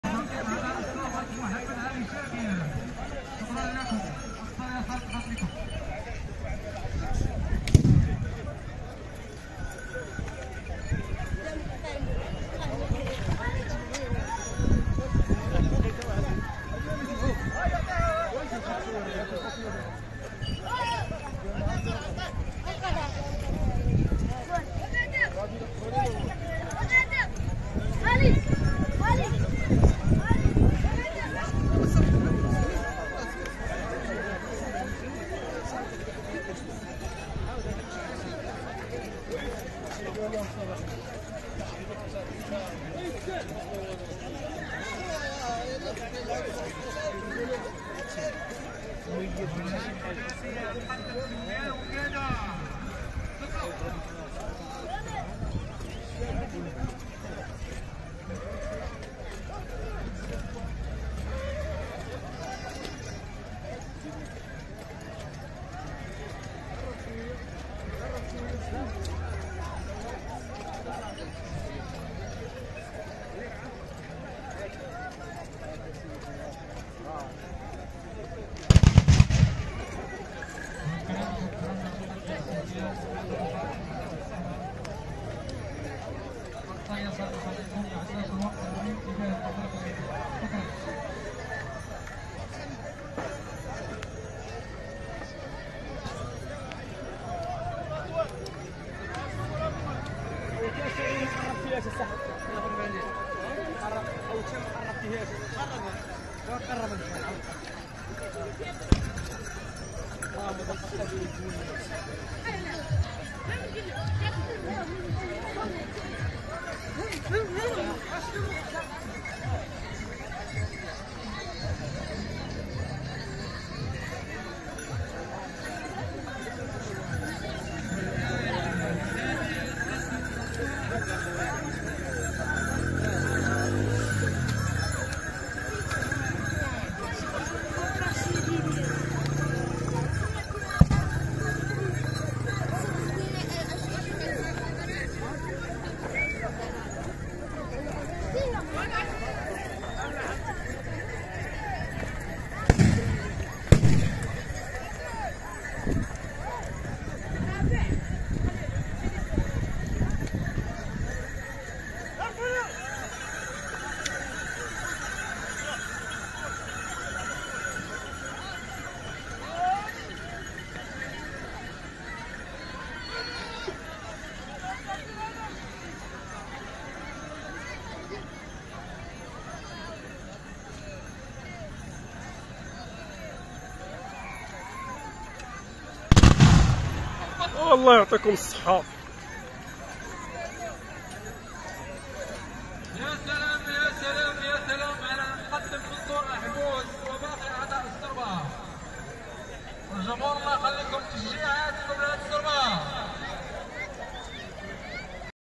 المقدم منصور أحبوس تألق و إبداع في مهرجان المݣرن لي التبوريدة
سربة المقدم منصور أحبوس